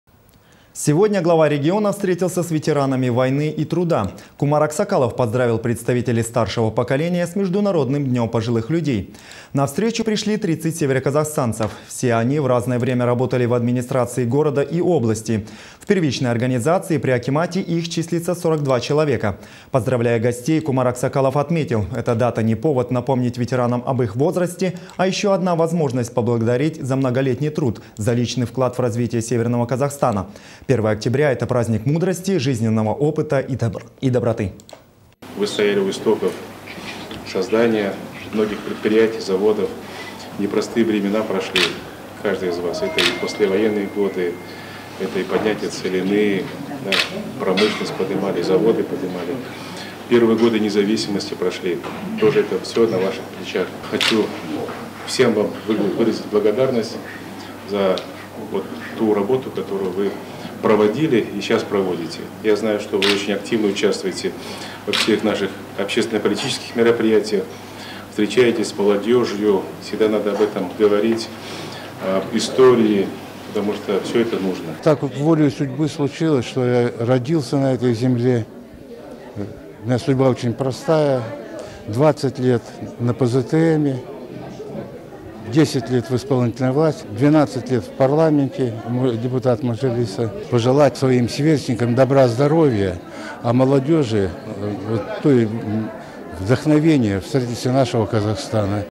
Сегодня глава региона встретился с ветеранами войны и труда
Сегодня глава региона встретился с ветеранами войны и труда. Кумар Аксакалов поздравил представителей старшего поколения с международным днём пожилых людей.